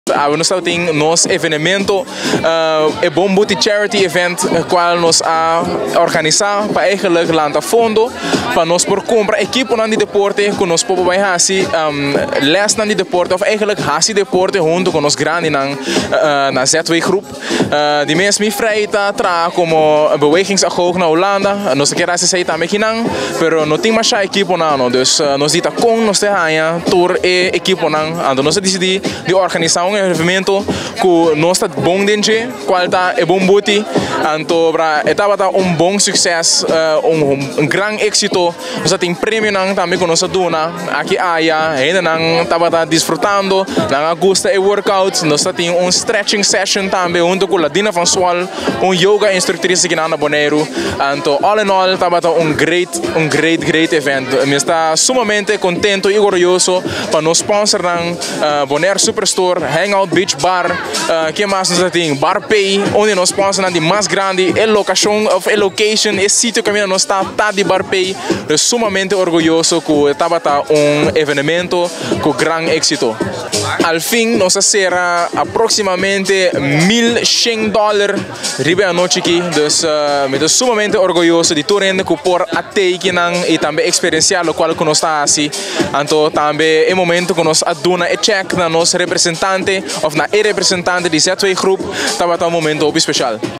Un evenementu kaminda ku nan a rekoudá fondo pa entrega na e representante di ZW Groep
Bon booty a organisá un evenementu kaminda ku  nan a rekoudá fondo pa entrega na e representante di ZW Groep. Christiaan Zweers ta amplia.